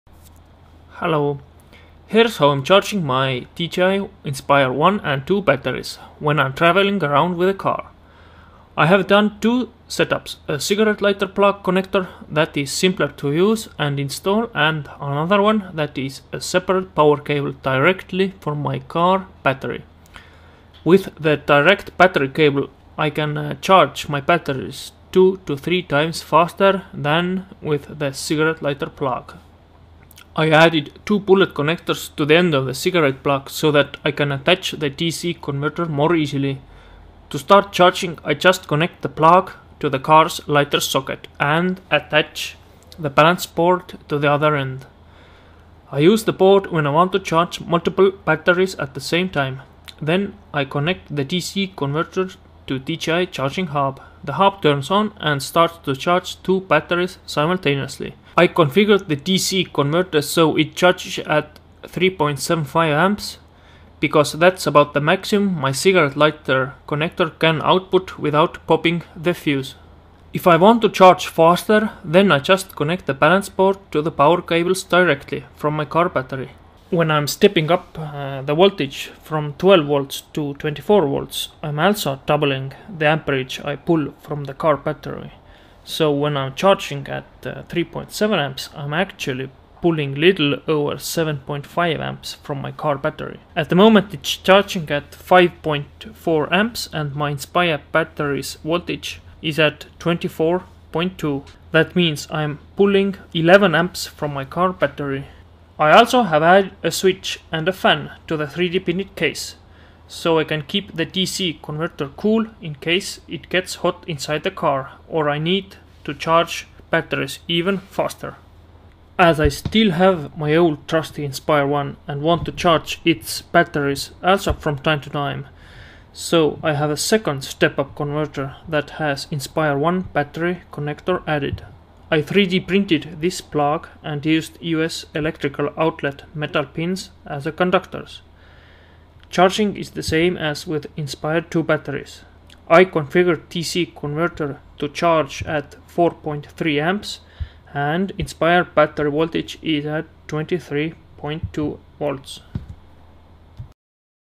Howto Charge DJI Inspire 2 & 1 batteries in a Car (Without an Inverter)
I made a short video of how i charge my DJI Inspire 2 and DJI Inspire 1 batteries when i am travelling with a car and don't have an electrical outlet nearby.

I can also change the voltage and current on the dc-to-dc converter to charge Phantom 3, Phantom 4/4 Pro, Matrice M100, M200, M600 and Mavic Pro batteries.

I am using LTC3780 based DC-to-DC converter
Voltmeter / Ammeter